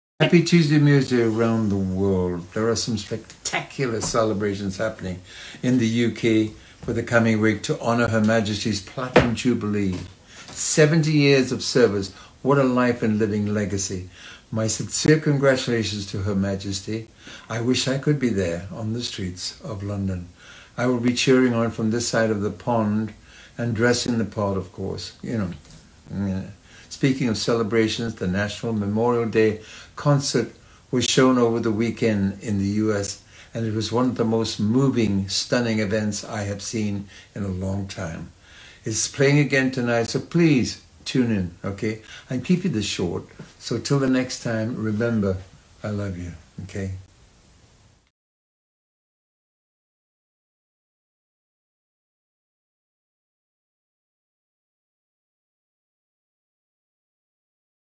The Queen's Platinum Jubilee (Tuesday Museday 142) - Engelbert Humperdinck Vlog
TuesdayMuseday 5/31/22 - Just a quick Tuesday Museday pop-in to chat about the upcoming PlatinumJubilee and the nationalmemorialdayconcert